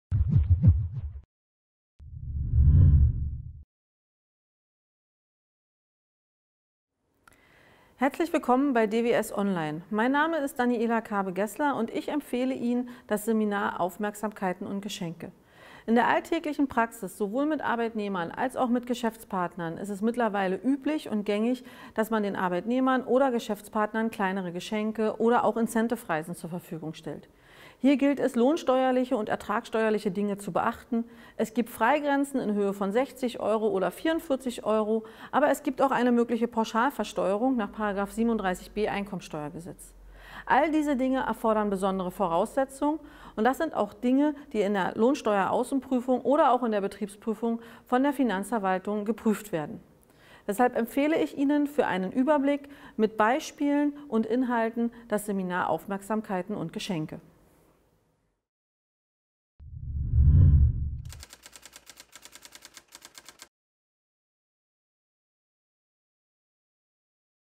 Aufmerksamkeiten und Geschenke
Daniela Karbe-Geßler RAin

Arbeitgeber motivieren ihre Arbeitnehmer unter anderem mit kleinen Geschenken oder Zuwendungen. Bei der steuerlichen Behandlung sind allerdings strenge Vorgaben und Freigrenzen zu beachten, damit diese kleinen Geschenke auch lohnsteuer-und sozialversicherungsfrei behandelt werden können. Tritt doch Lohnsteuerpflicht ein, so können die Geschenke gegebenenfalls pauschal versteuert werden. Die einzelnen Voraussetzungen werden im Seminar erläutert.